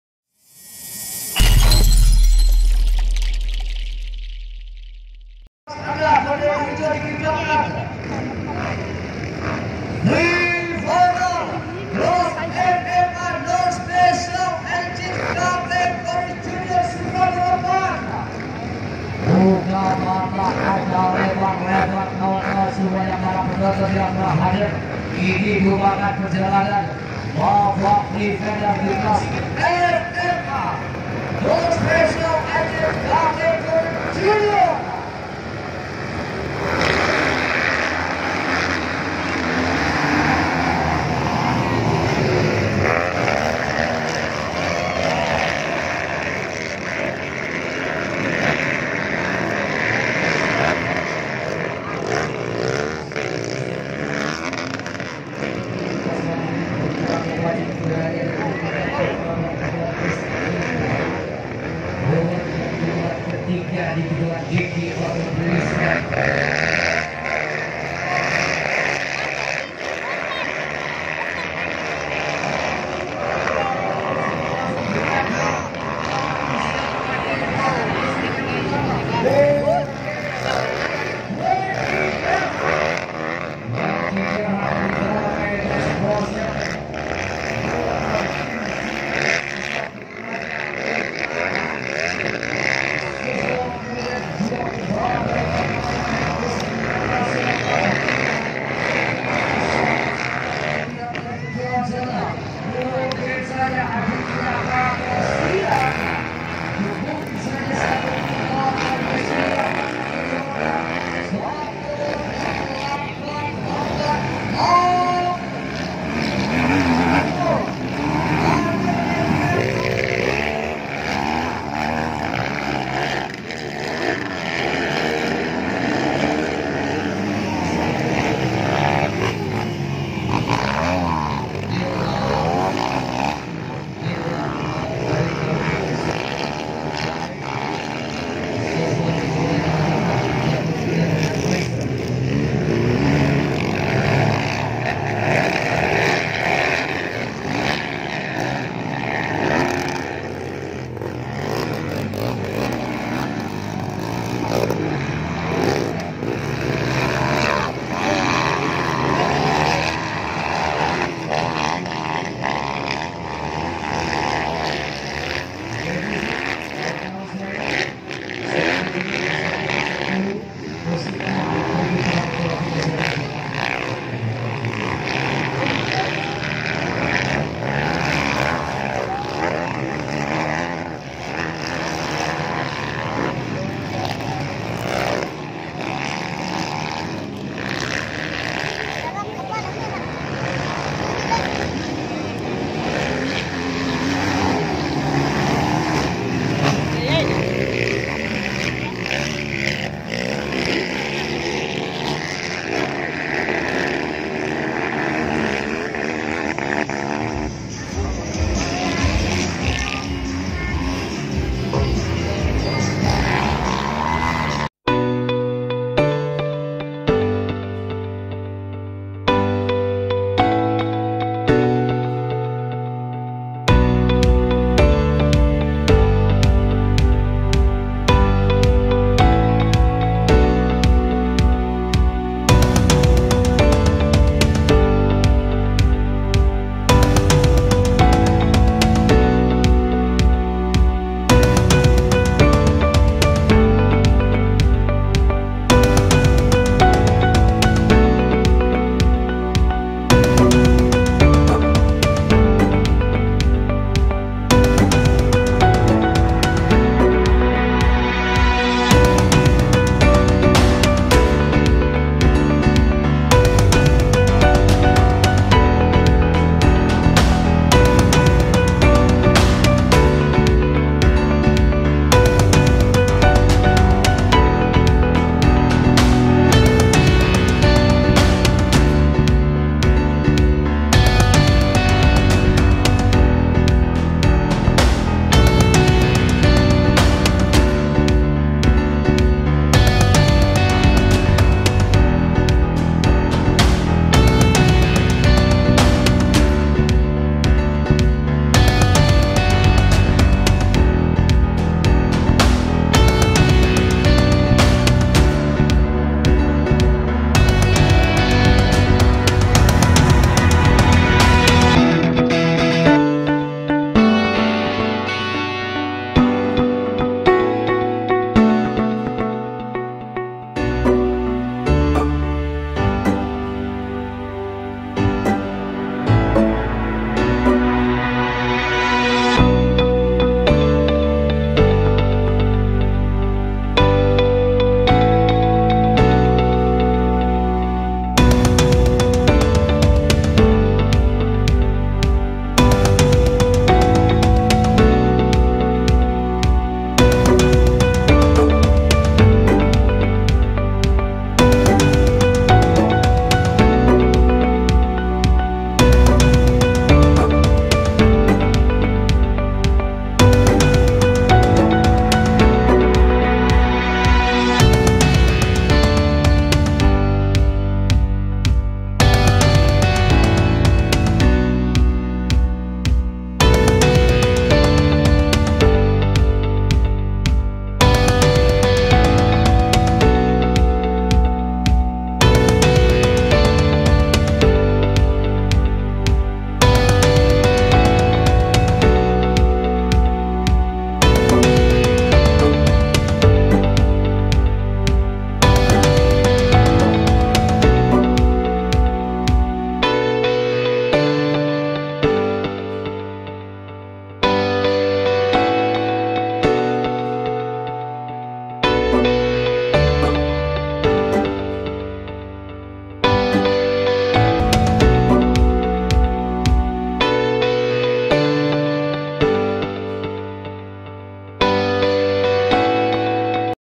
NASIB SIAL YANG DIALAMI PEMBALAP DI SIRKUIT BALAP BOTTOLAMORO SJP FINAL KEJURDA GRASSTRACK MOTOCROSS
Buat seorng pembalap bukan sirkuit yang mereka takuti tapi yang pling mereka takuti adalah disaat balapan mesin motor yg di kendarai oleh pembalap mati mesin ,. itulah yang terjadi pada pembalap di vidio ini,. bahkan putran pertama saja belum setengah putran,.

Wassalam
Riani_amg